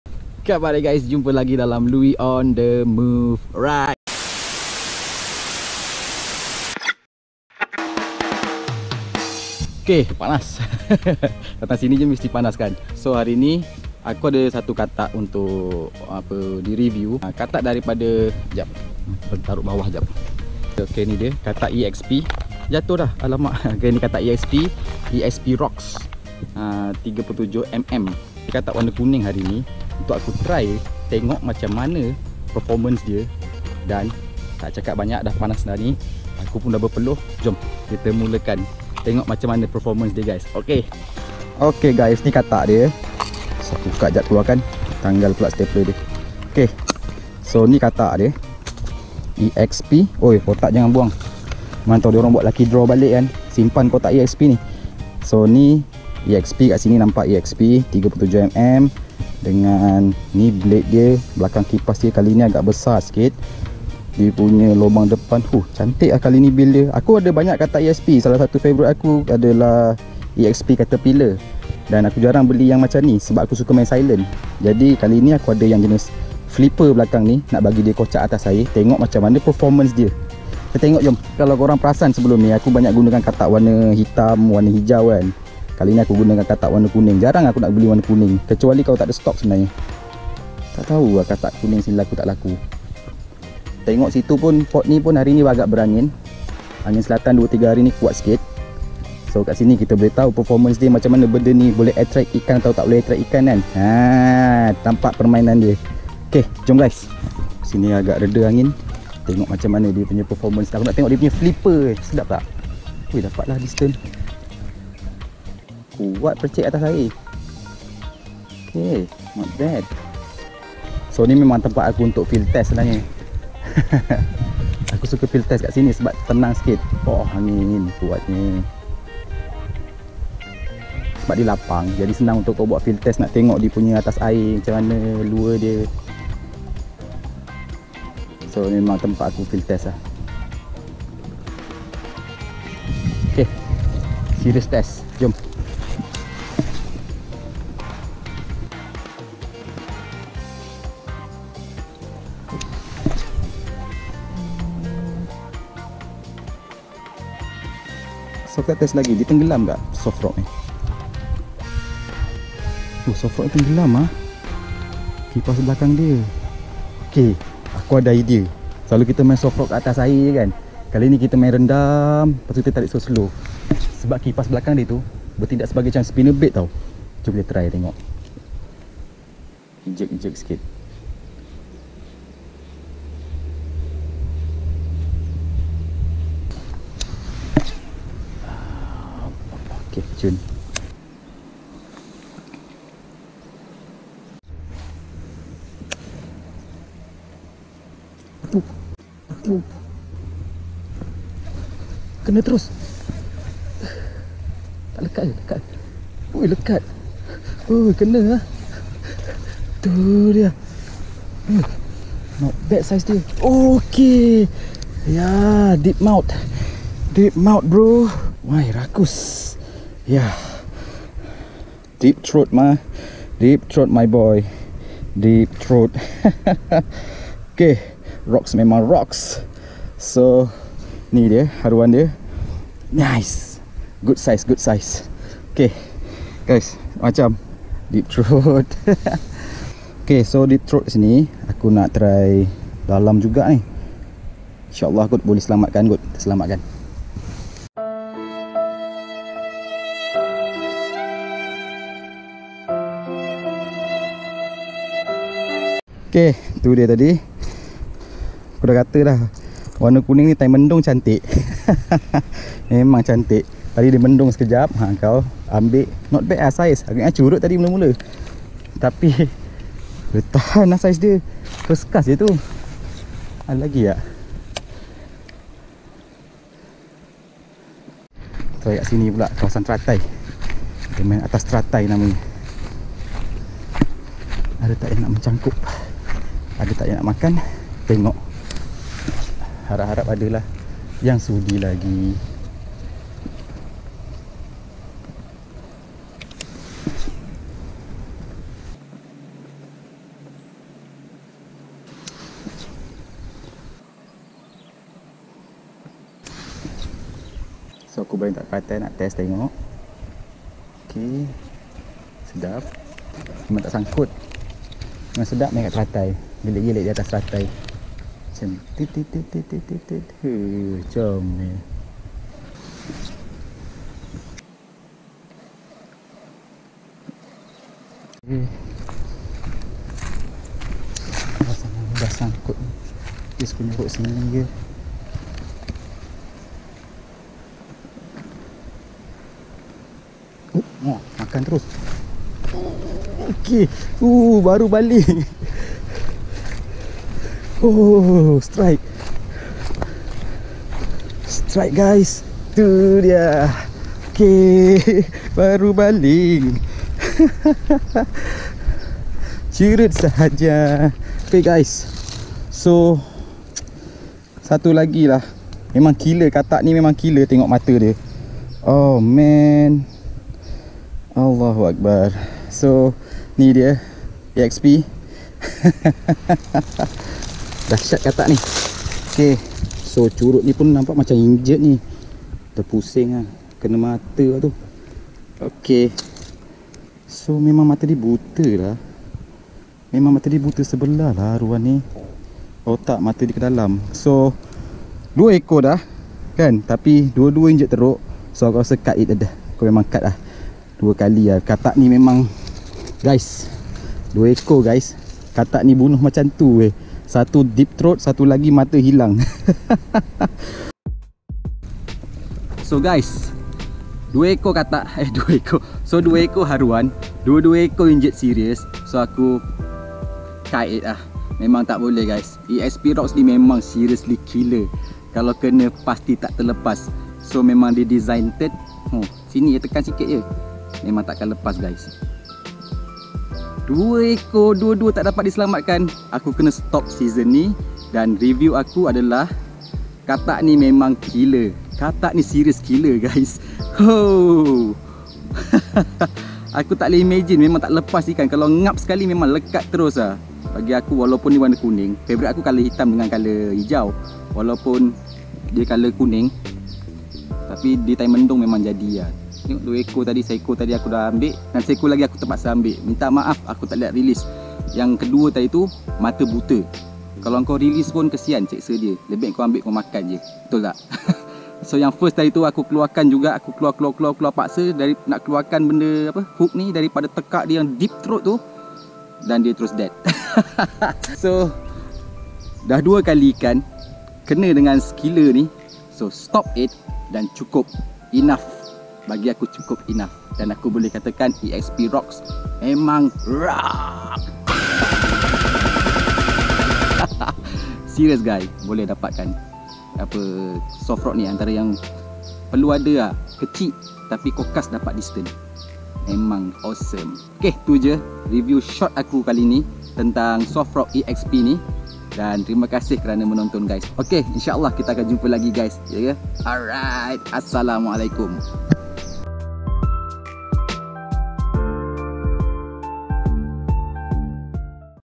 | Real Killer Softfrog : EXP Rox Soft Frog | Ep. 50 | Casting Ikan Haruan @ Gabus |
Assalamualaikum gais..
Untuk vlog pendek kali ni Luie berkesempatan untuk mencuba soft frog keluaran EXP.. EXP Rox Frog.. Apa yang boleh Luie katakan tentang soft frog ni cuma 1 perkataan sahaja.. WOW!!.. Hahahaha..

Casting set
Reel : Abu Garcia Revo ALT
Rod :  Storm Trickster 4-10lb
Line : Tokuryo Prox4 16lb
Lure : EXP Rox Frog Softfrog